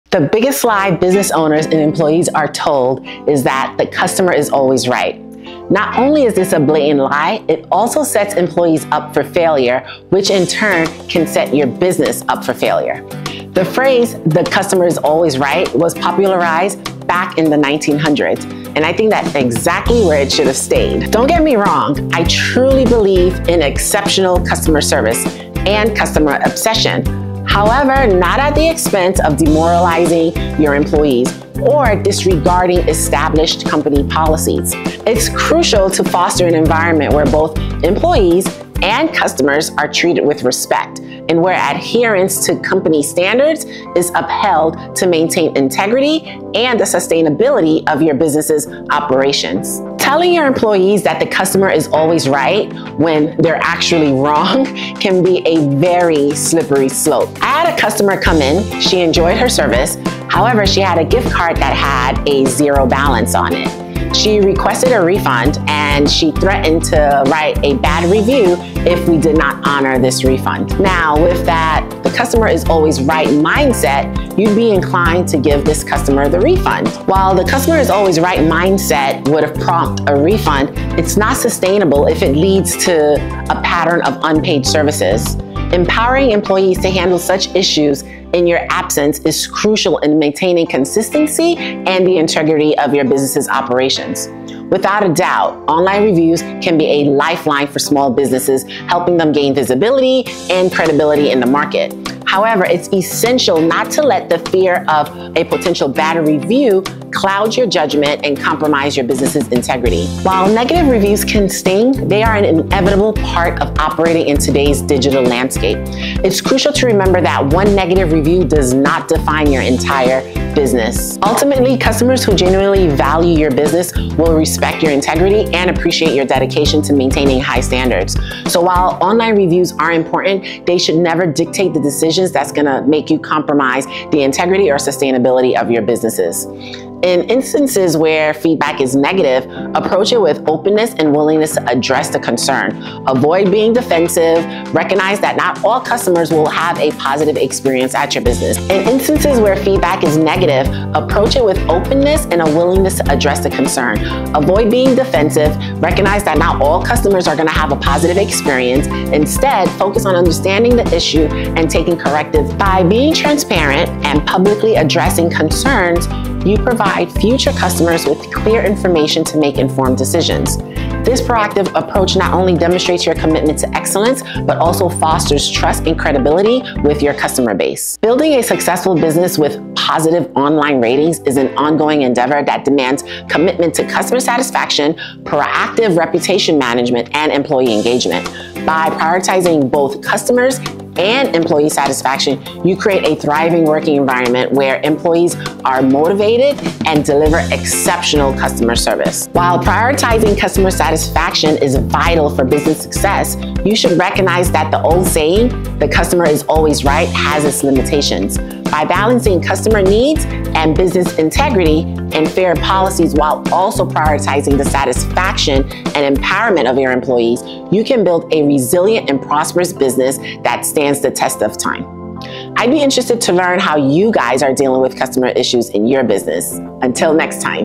Hmm..Is the Customer Always Right?
Is the customer always right?  Get ready for an exciting adventure into the world of business and entrepreneurship! In this video, we'll explore a famous saying: 'The customer is always right.' But is this always true? Join us as we uncover surprising stories and learn about important ideas like integrity, fairness, and customer satisfaction. Discover how successful businesses balance the needs of customers with smart business practices. You'll be inspired to think like an entrepreneur and make smart choices in your own endeavors. Let's dive in and unlock the secrets of business success together!